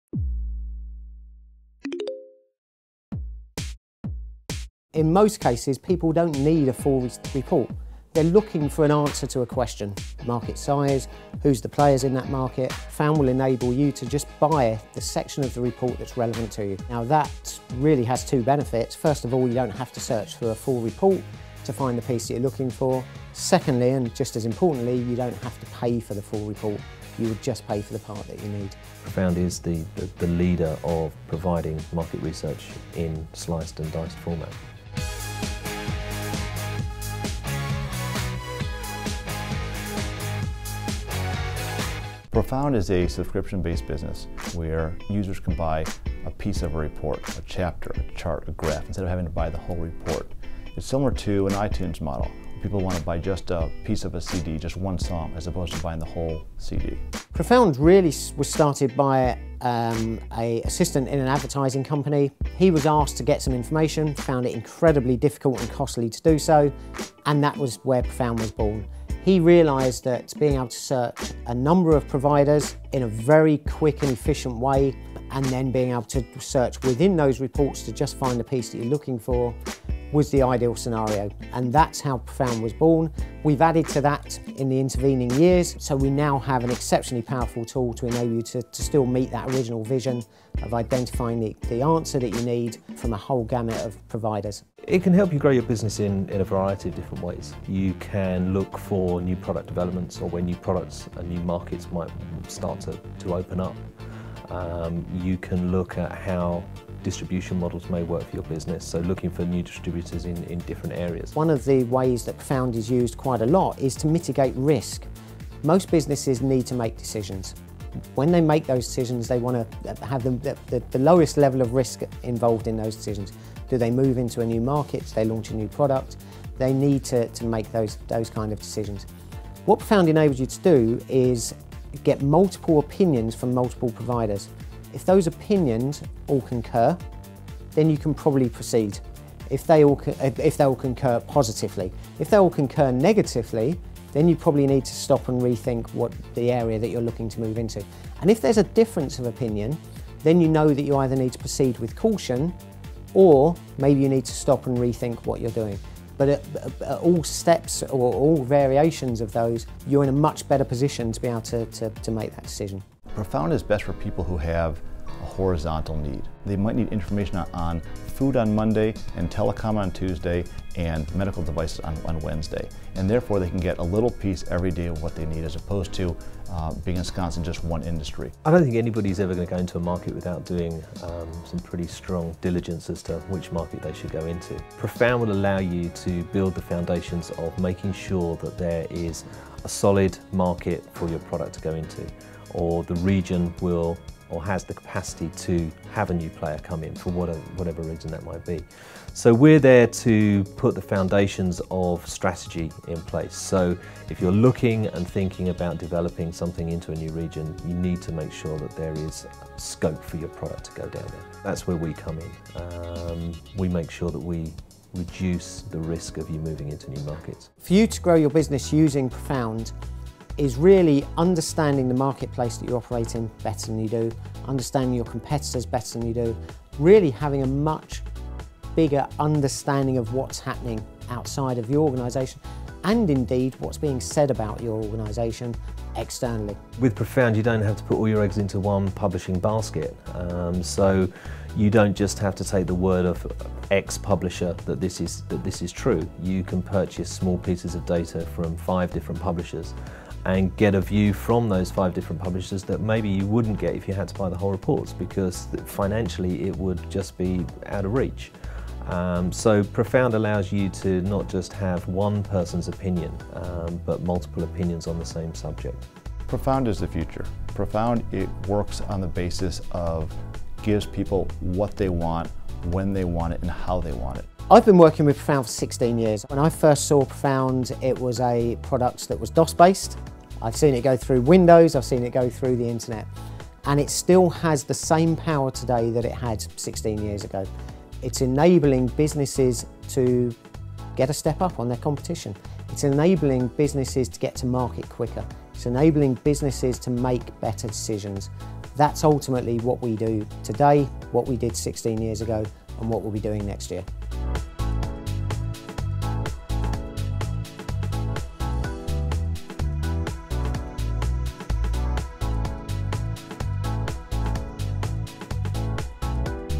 What is profound?
Profound is the premier corporate business intelligence service -- a time-saving, cost-effective, user-friendly tool helping thousands of companies globally find the market research they need to stay ahead.

In this video, learn how Profound can help you grow your business, with MarketResearch.com CEO Rob Granader, General Manager Ray King and Sales Manager Jason Ward.